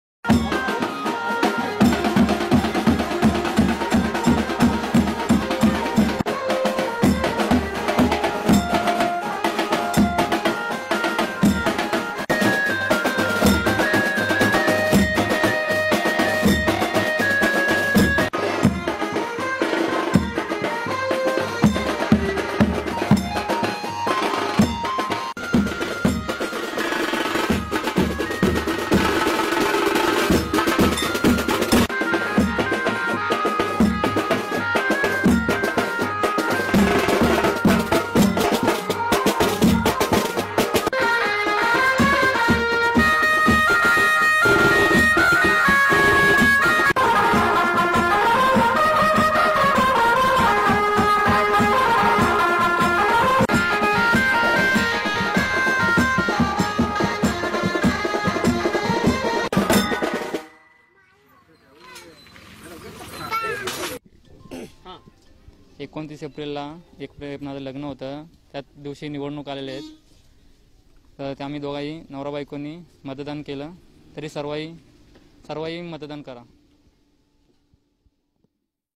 लग्न लागताच, नवरा-नवरी ढोल ताशांच्या गजरात पोहोचले मतदान केंद्रात बजावला मतदानाचा हक्क
लग्न लागताच, नवरा-नवरी
ढोल ताशांच्या गजरात पोहोचले मतदान केंद्रात
बजावला मतदानाचा हक्क